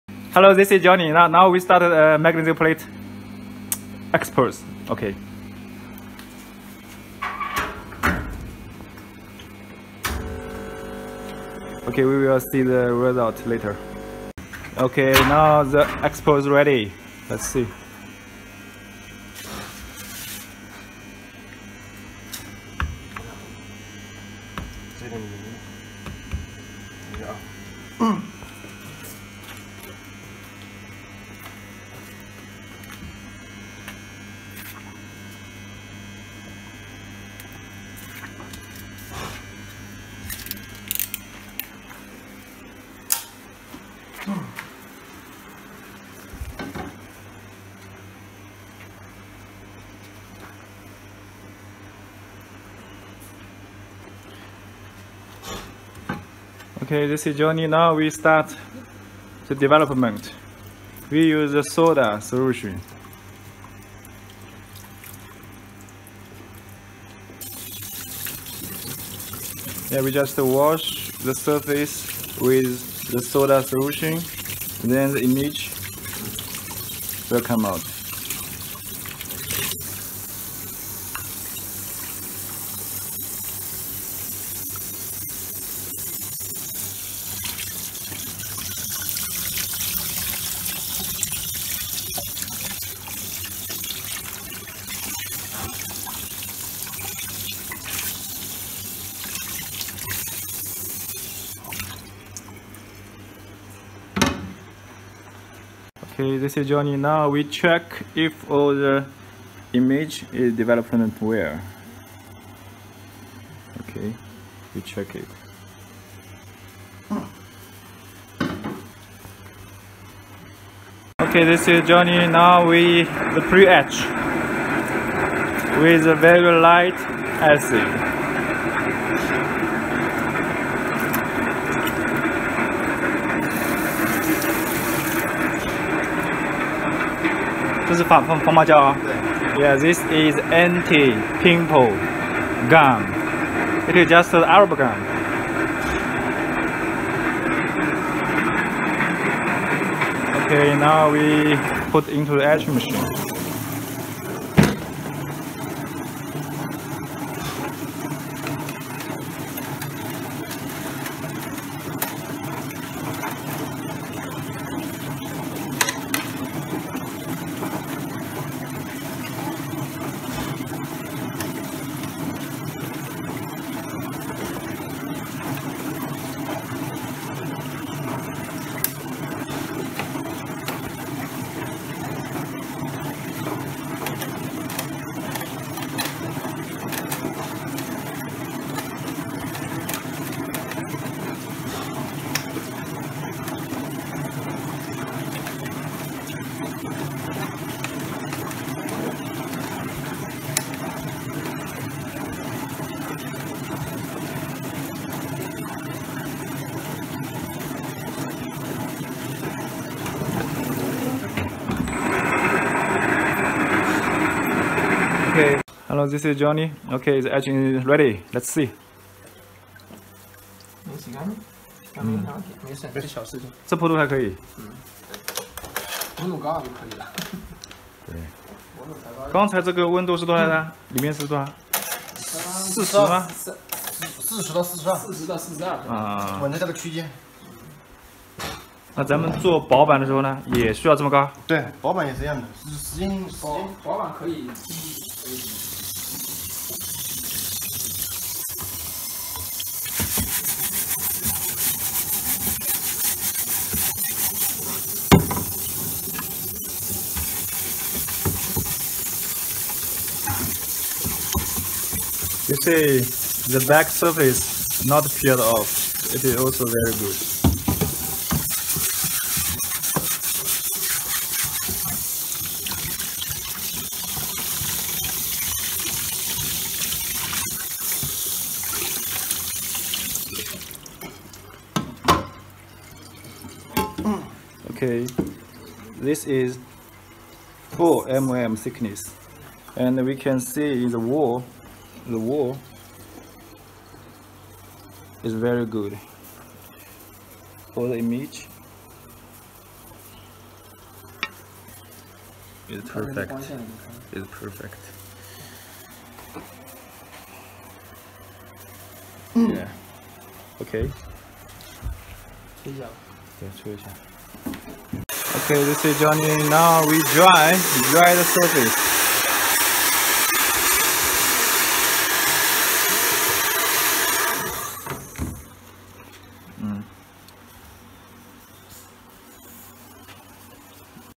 SGZ Coated Magnesium Plate etching video
SGZ Hydro coated magnesium plate.
Whole etching video.
Both quality and price super good!